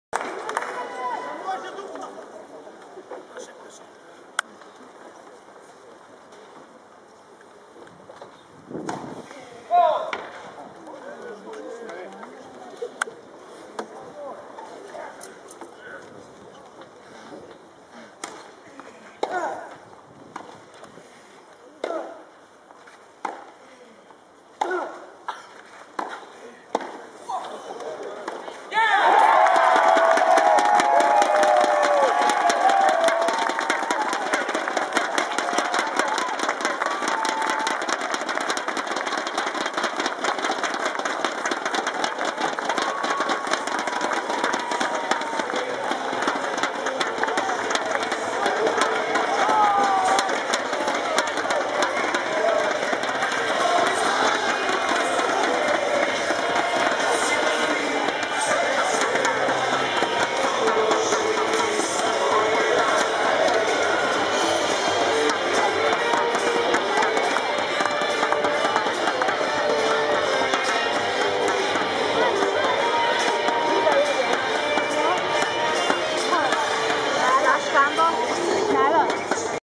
Andy Murray in Umag Davis Cup 3rd Day
Croatia - GB Davis Cup play-off tie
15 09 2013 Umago